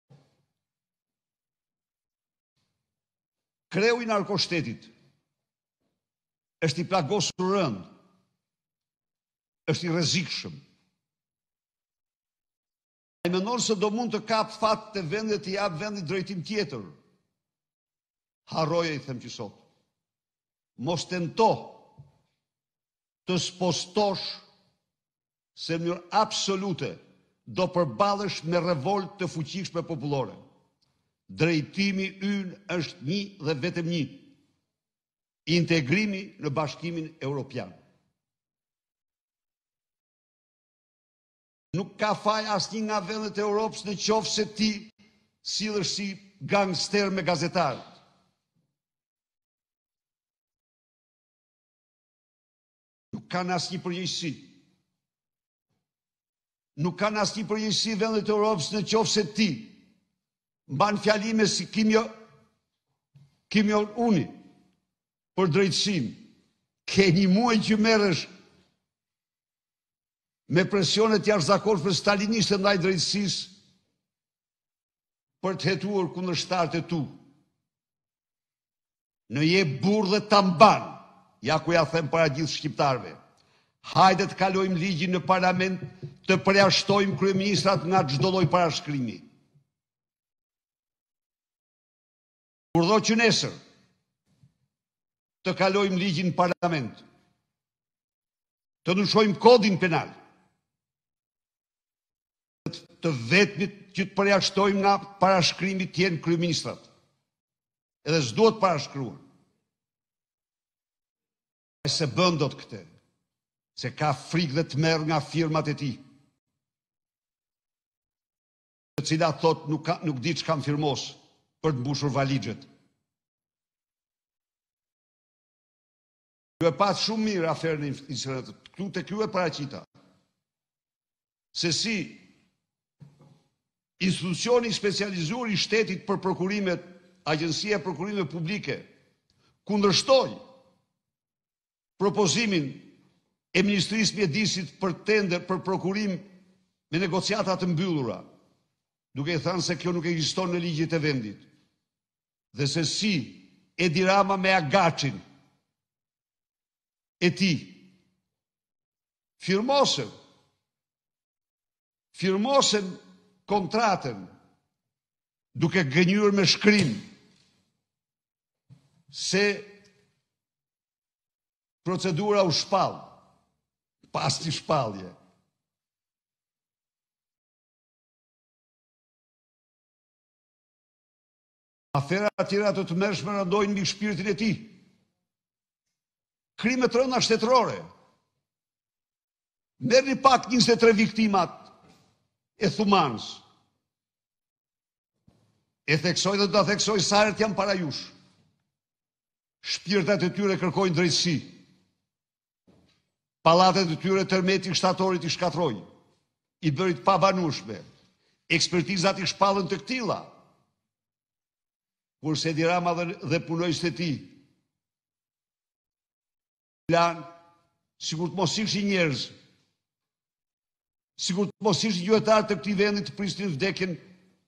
Berisha ftesë Ramës: Nëse ta mban, hajde të kalojmë ligjin në parlament që përjashton kryeministrat
reportTV lajmeshqip lajme Ish kryeministri Sali Berisha i ka bërë ftesë Edi Ramës që të kalojnë ligjin në parlament për të përjashtuar kryeministrat nga çdo lloj parashkrimi, por thotë se nuk e bën këtë dhe e krahason me Kim Jong Un për deklaratat që i referohen drejtësisë.

"I them sot mos tento të spostosh vëmendjen se do përballesh me revoltë popullore, drejtimi ynë është vetëm një integrimi në BE. Nuk ka faj asnjë vend në BE nëse ti sillesh si gangster me gazetarët. Nuk kanë përgjegjësi vendet e Evropës kur ai mban qëndrime si Kim Jong-un ndaj Drejtësisë për të hetuar kundërshtarët e tij.

Në je burrë dhe ta mban hajde të kalojmë ligjin në parlament, të përjashtojmë kryeministrat nga çdo lloj parashkrimi, të ndryshojmë kodin penal. Ti s’e bën dot këtë! Se nuk e di ç’ke firmos për të mbushur valixhet. Do ti jap një paralajmërim të fortë, nuk mund të luajë me këtë vend. Vasal i Aleksandër Vuçiçit. Le ta provojë po të dojë", tha Berisha.